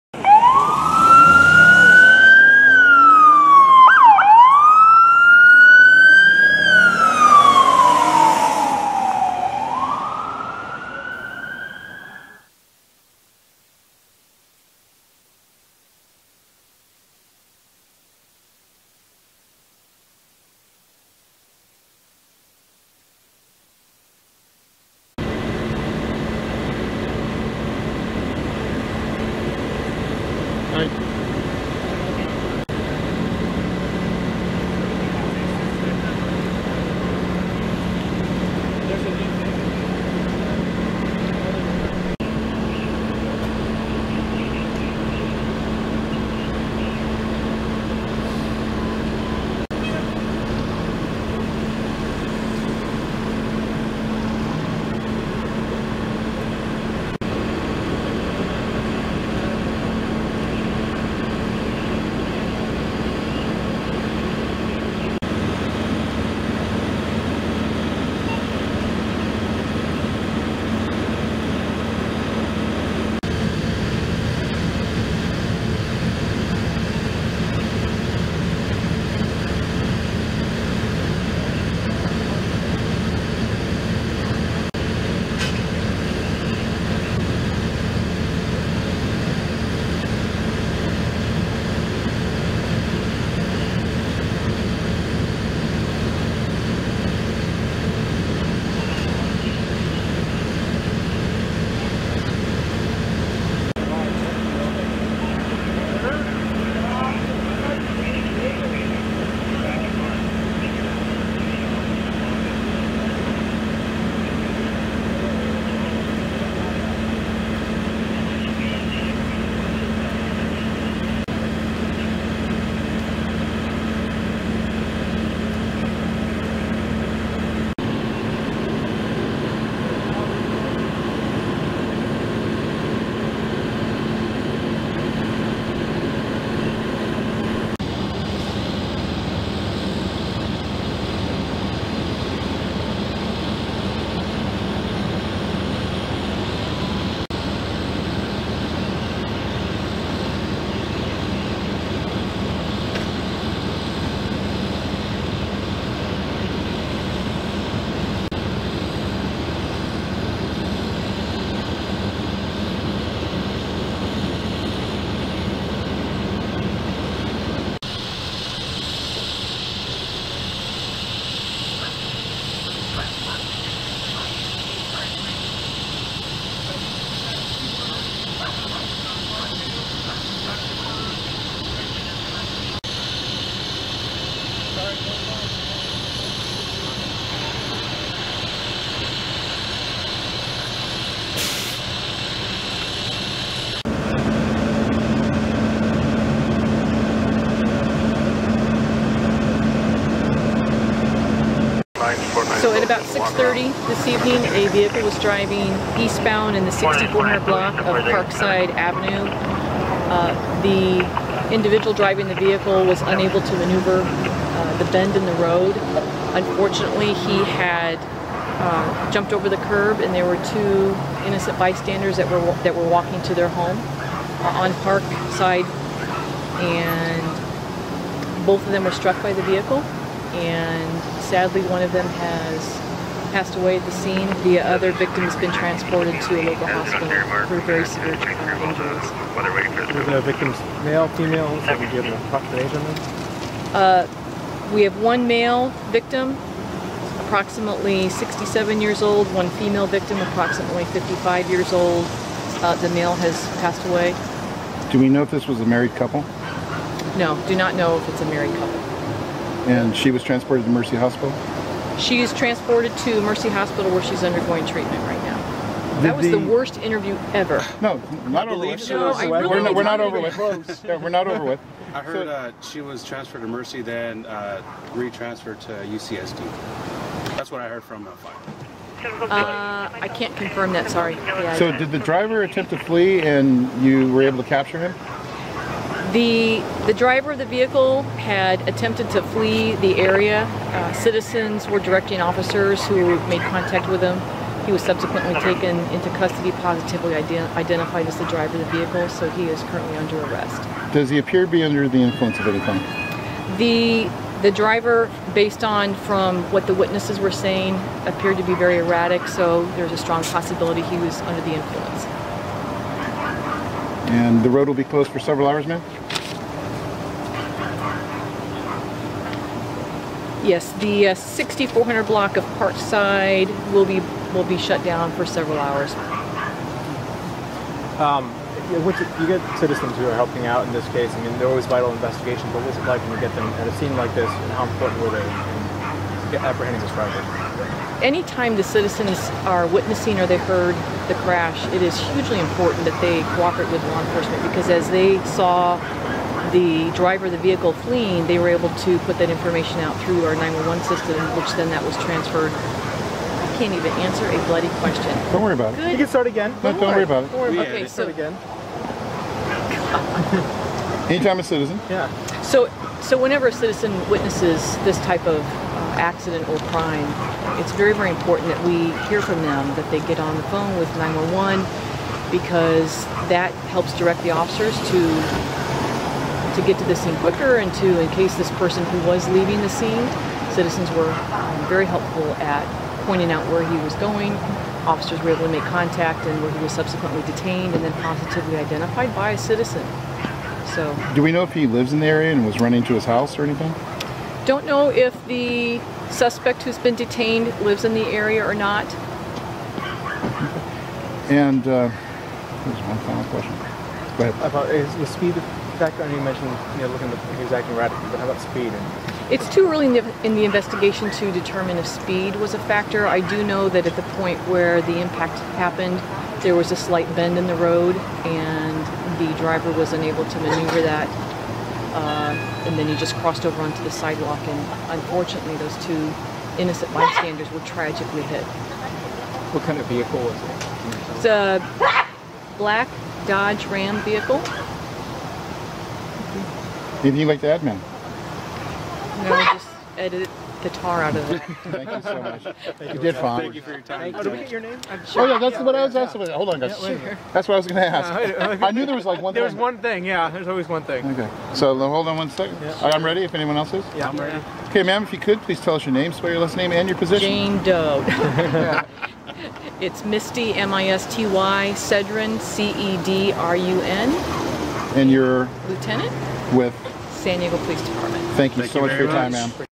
Fatal Hit & Run In Paradise Hills 2/3/2018
Date/Time: 2/3/2018 - 6:25pm
City: San Diego
Location: 6402 Parkside Ave. (Paradise Hills)
Interview: Lieutenant Misty Cedrun (San Diego Police)
Incident Details:  Full details can be found at 3:22 minute mark of the video.

Video made in collaboration with 911 NEWS VIDEO.